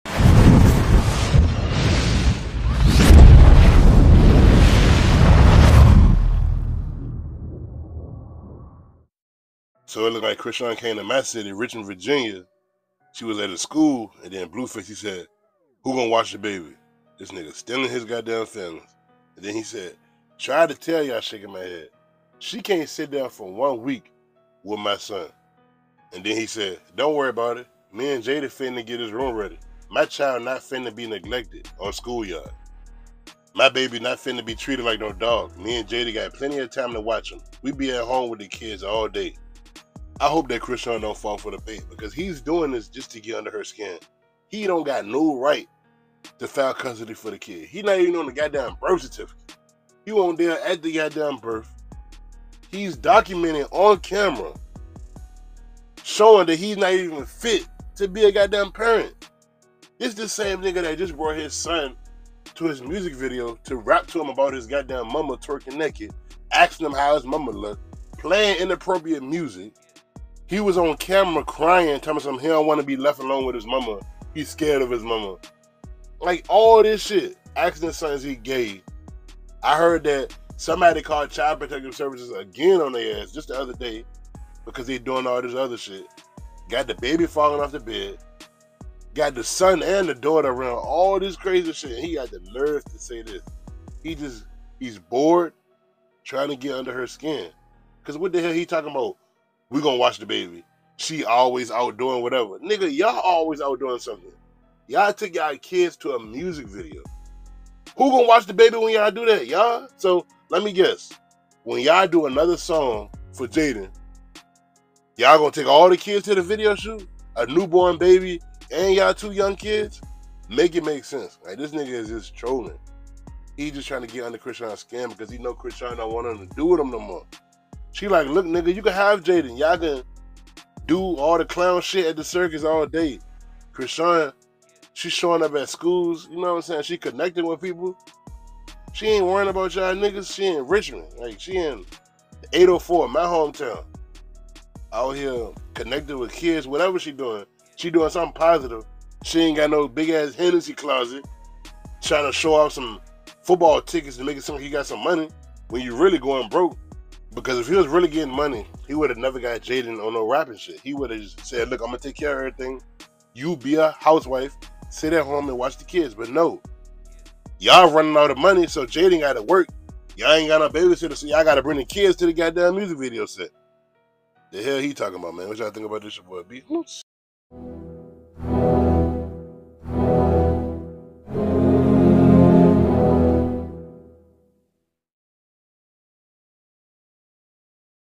Blueface Goes On A Emotional Rant After Chrisean Goes To Richmond Va! "Jaidyn Got Time To Watch Him"
he's so sad 😂